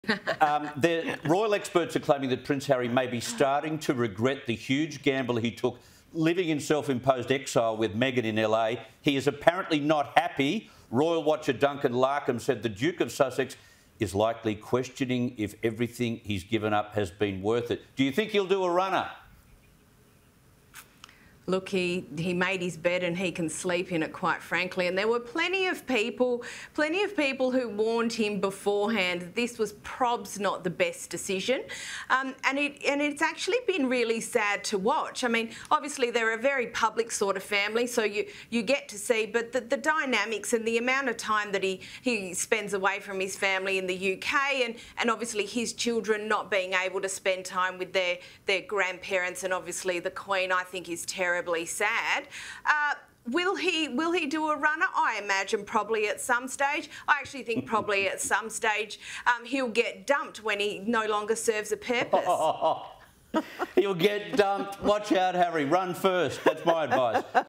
Prince Harry may be starting to regret 'huge gamble' living in 'self-imposed exile'
Sky News host Chris Smith says royal experts are claiming Prince Harry may be starting to “regret the huge gamble” he took by living in “self-imposed exile” in America with his wife, Meghan Markle.

“He’s apparently not happy,” he said.

“Royal watcher Duncan Larcombe said the Duke of Sussex is likely questioning if everything he's given up has been worth it.”